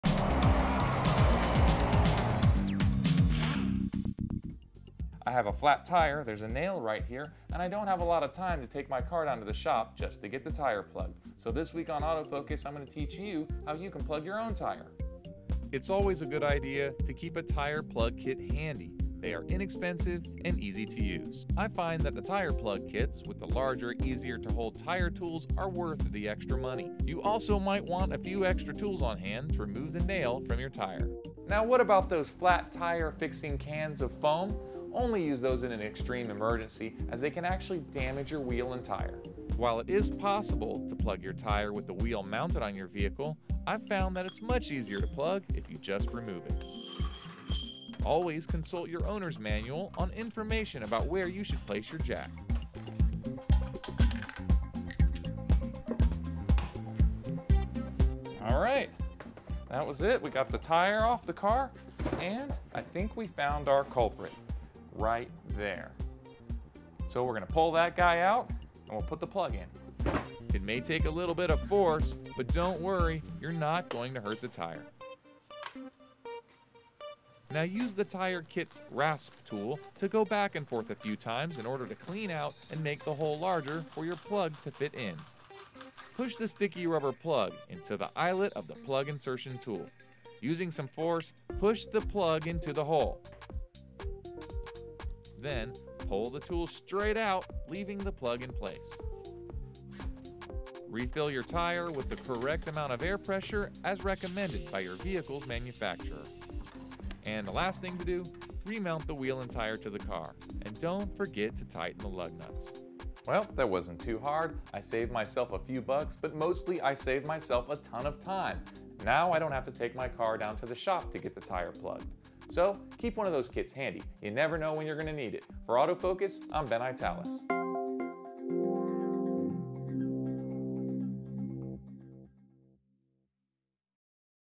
Autofocus -You can plug your own tire
Tired of waiting for hours in a repair shop just to have your flat tire repaired. Automotive Journalist, Ben Eytalis demonstrates how you can repair that hole in your car's tire quickly and for just a few bucks.